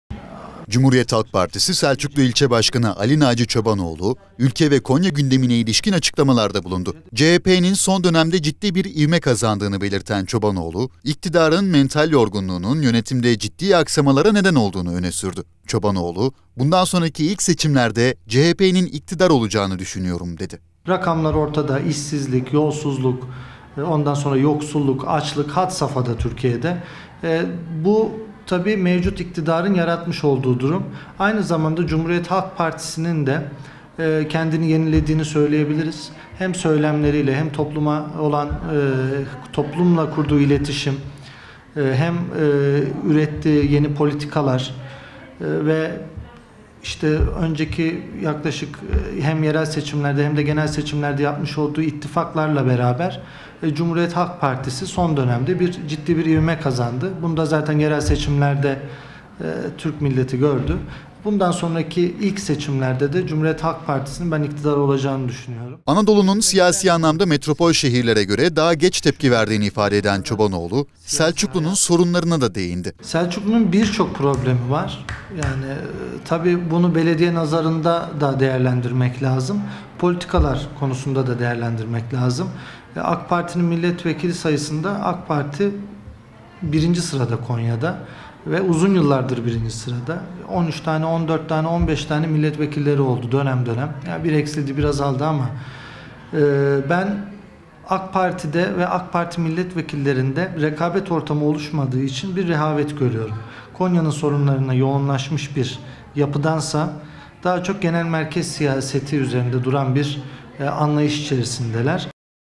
“Türkiye bir kişinin iki dudağının arasında”
CHP Selçuklu İlçe Başkanı Ali Naci Çobanoğlu, CHP’nin vatandaş nezdinde ciddi bir ivme yakaladığını vurgulayarak Türkiye’nin önemli sorunlarla karşı karşıya olduğunu belirtti